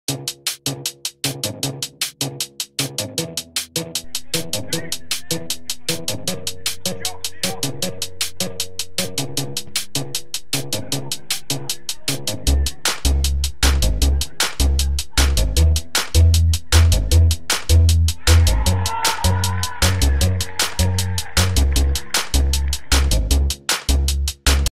SESTŘIH ÁČKO I Motorlet B - Tempo 1:0
Pražská teplárenská přebor, 29. kolo, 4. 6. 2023
FK Motorlet Praha B - FC Tempo Praha 1:0 (1:0)
Branka: Pokorný